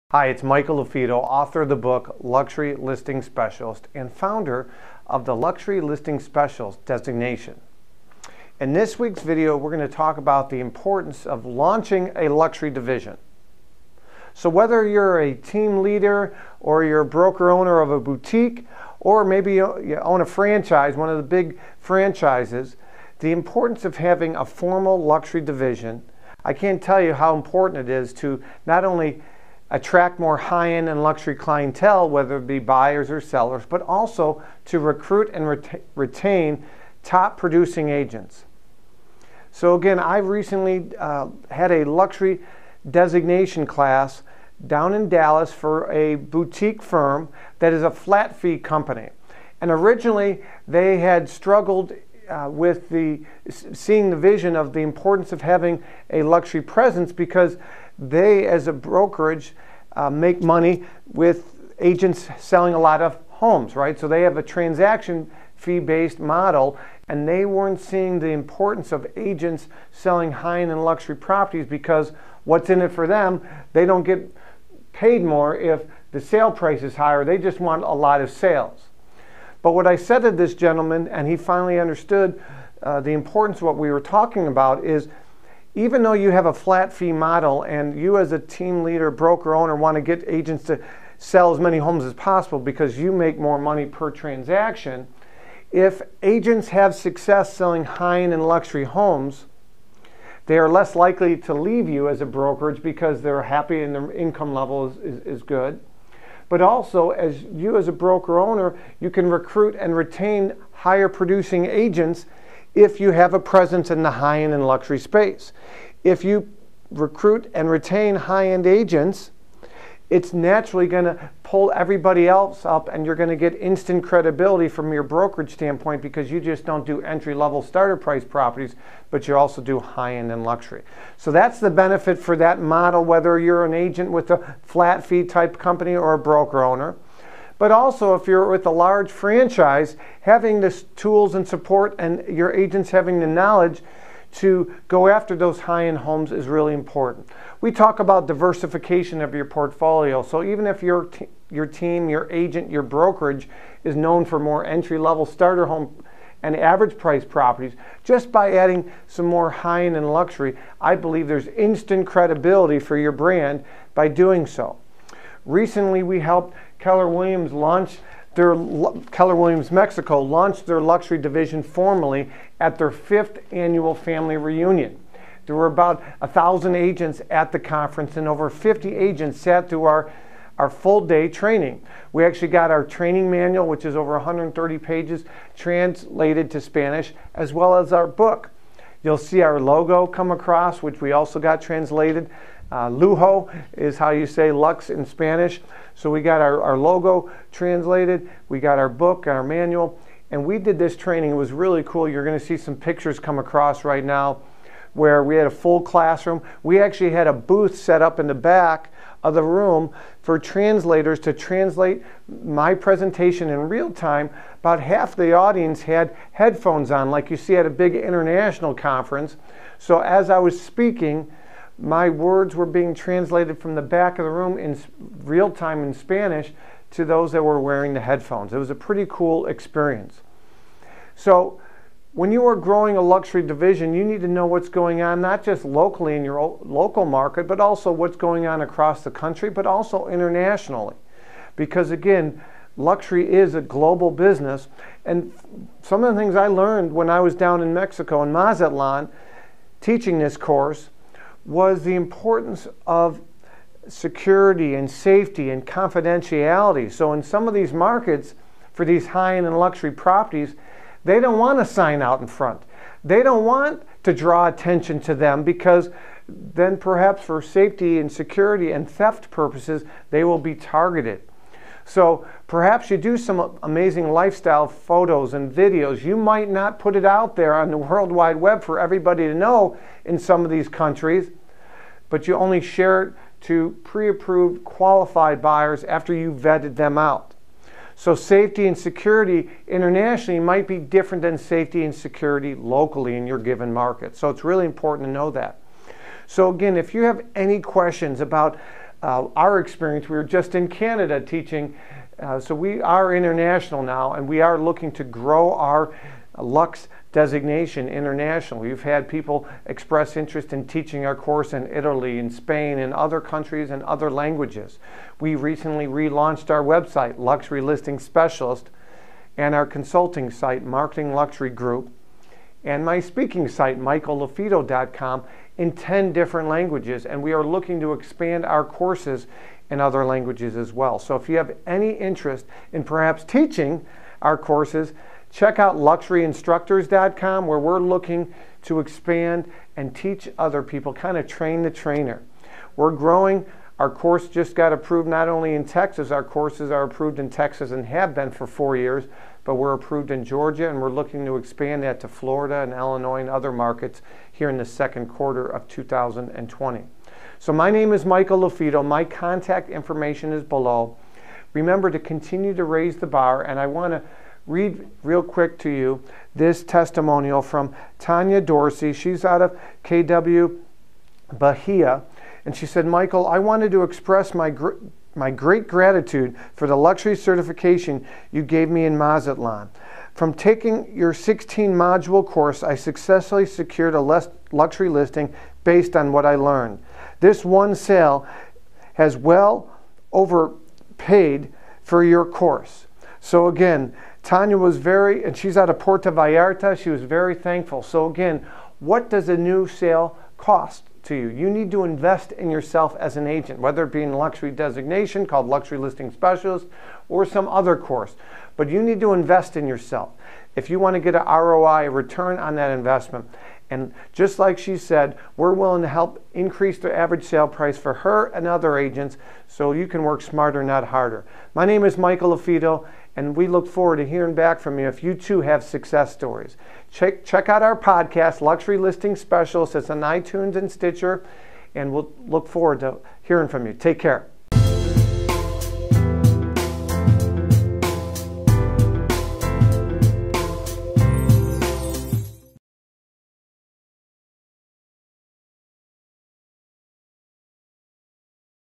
The Importance Of Launching A Luxury Division | Luxury Listing Specialist
Whether you’re a team leader, a broker-owner of a boutique, or a big franchise owner, the importance of having a luxury division that offers agents the best tools and resources can’t be overstated. Having a strong luxury division will help attract top-producing agents, but it will also boost the credibility of a brokerage's brand. To find out more details and to hear highlights from how we helped with the launch of Keller Williams Mexico’s luxury division, watch my latest video.

Michael LaFido
MarketingLuxuryGroup.com
LuxuryListingSpecialist.com
LuxuryListingPodcast.com